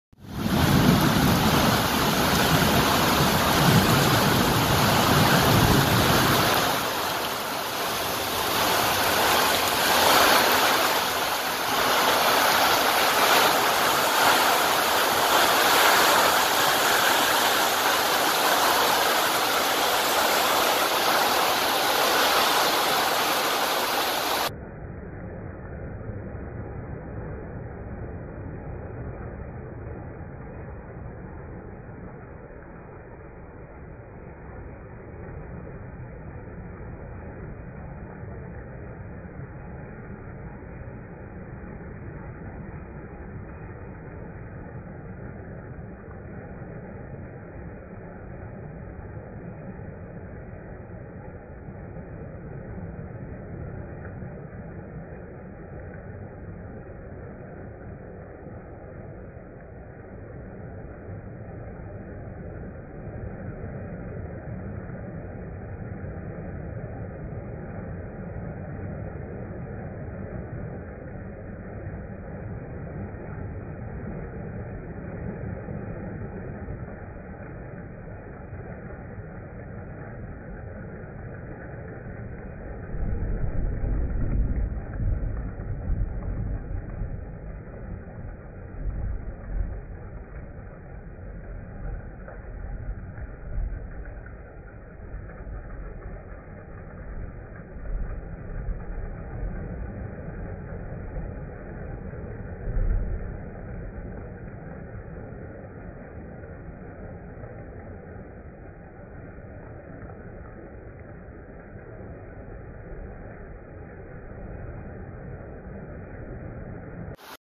Tennessee Rain
Nice storm rain on a tin roof